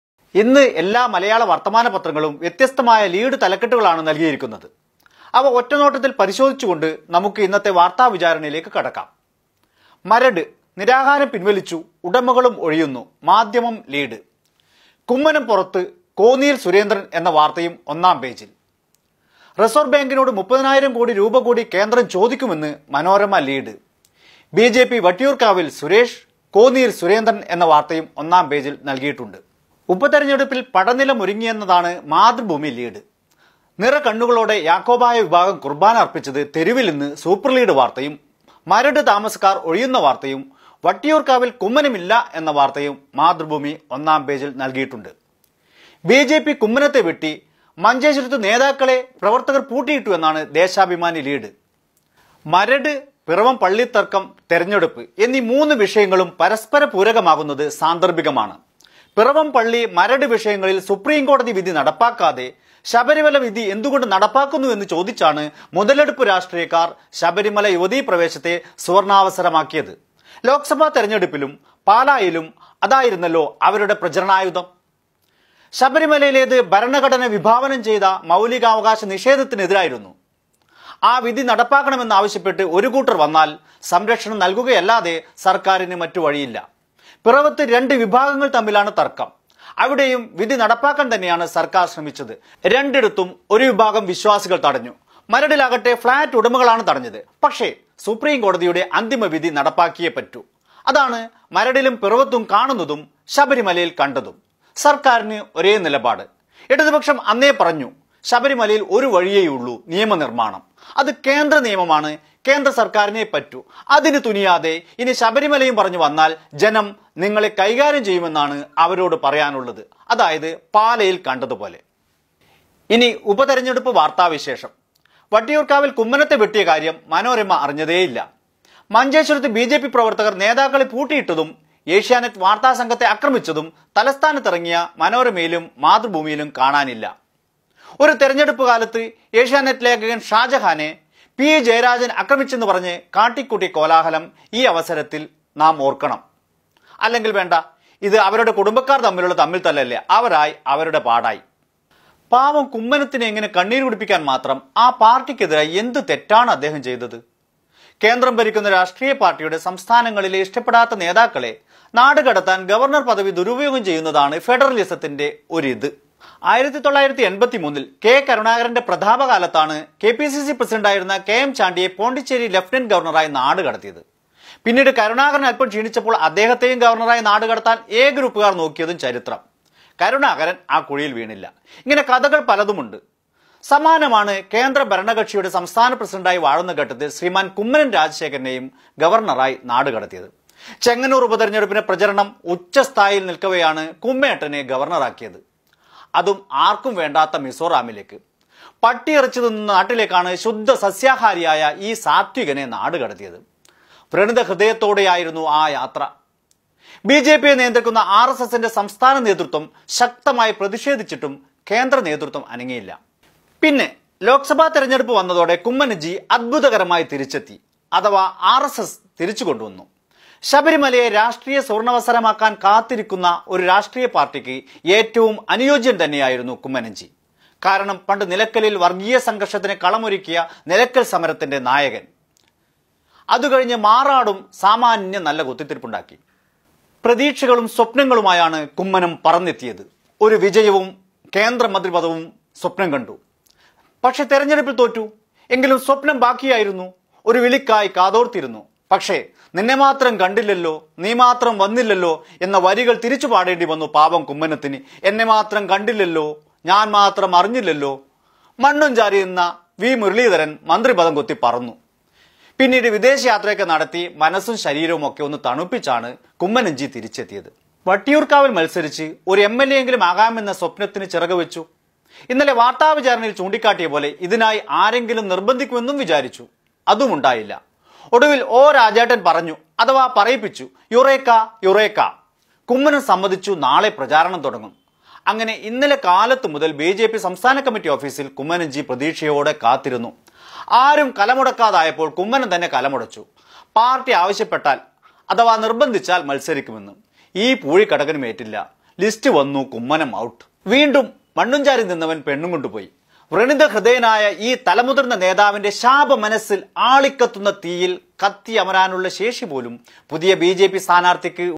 ഇന്ന്‌ എല്ലാ മലയാള വർത്തമാന പത്രങ്ങളും വ്യത്യസ്‌തമായ ലീഡ്‌ തലക്കെട്ടുകളാണ്‌
ഇന്ന്‌ എല്ലാ മലയാള വർത്തമാന പത്രങ്ങളും വ്യത്യസ്‌തമായ ലീഡ്‌ തലക്കെട്ടുകളാണ്‌ | നൽകിയത്‌. അവ ഒറ്റ നോട്ടത്തിൽ പരിശോധിച്ച്‌ നമുക്ക്‌ ഇന്നത്തെ വാർത്താ വിചാരണയിലേക്ക്‌ കടക്കാം. മരട്‌, നിരാഹാരം പിൻവലിച്ചു ഉടമകളും ഒഴിയുന്നു‐മാധ്യമം ലീഡ്‌, കുമ്മനം പുറത്ത്‌ കോന്നിയിൽ സുരേന്ദ്രൻഎന്ന വാർത്തയും ഒന്നാം പേജിൽ. റിസർവ്‌ ബാങ്കിനോട്‌ 30,000 കോടി രൂപ കൂടി കേന്ദ്രം ചോദിക്കുമെന്ന്‌ മനോരമ ലീഡ്‌, ബിജെപി, വട്ടിയൂർക്കാവിൽ സുരേഷ്‌, കോന്നിയിൽ സുരേന്ദ്രൻഎന്ന വാർത്തയും ഒന്നാം പേജിൽ നൽകി. ഉപതെരഞ്ഞെടുപ്പിൽ പടനിലമൊരുങ്ങിയെന്നതാണ്‌ മാതൃഭൂമി ലീഡ്‌. നിറകണ്ണുകളോടെ യാക്കോബായ വിഭാഗം കുർബ്ബാന അർപ്പിച്ചത്‌ തെരുവിൽ എന്ന സൂപ്പർ ലീഡ്‌ വാർത്തയും മരട്‌ താമസക്കാർ ഒഴിയുന്ന വാർത്തയും വട്ടിയൂർക്കാവിൽ കുമ്മനമില്ല എന്ന വാർത്തയും മാതൃഭൂമി ഒന്നാം പേജിൽ നൽകിയിട്ടുണ്ട്‌. ബജൈപി കുമ്മനത്തെ വെട്ടി, മഞ്ചേശ്വരത്ത്‌ നേതാക്കളെ പ്രവർത്തകർ പൂട്ടിയിട്ടു എന്നാണ്‌ ദേശാഭിമാനി ലീഡ്‌.